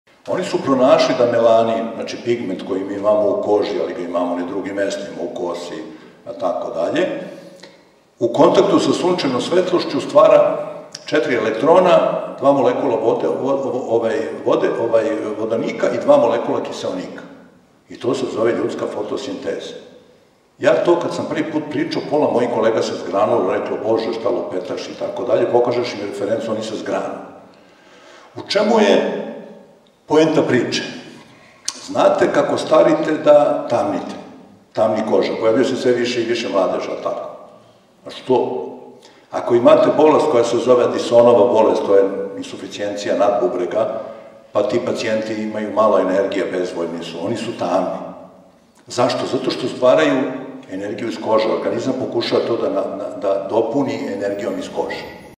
Branimir Nestorović - ZDRAVLJE KOŽA I ENERGIJA - Ljudska fotosinteza
Pojedine bolesti uzimaju energiju iz kože, a prof. dr Branimir Nestorović objašnjava detalje.. O melaninu, energiji, koži…
🎬 Ovo je kratak odlomak, celo predavanje pogledajte na Youtube kanalu Zlatno doba ☀️ pod nazivom “Garjajev i Tesla - Gledanje u Sunce - dr Branimir Nestorović